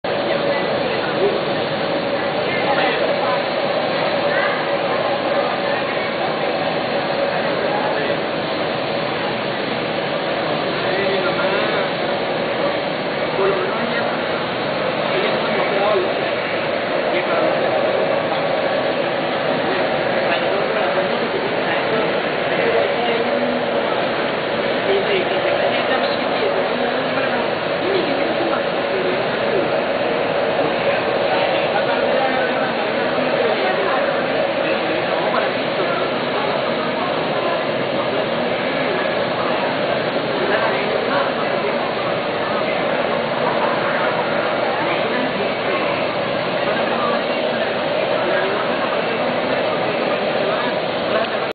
CONVIASA A340 despegue desde Maiquetia
despegue desde maiquetia con destino Damasco Teheran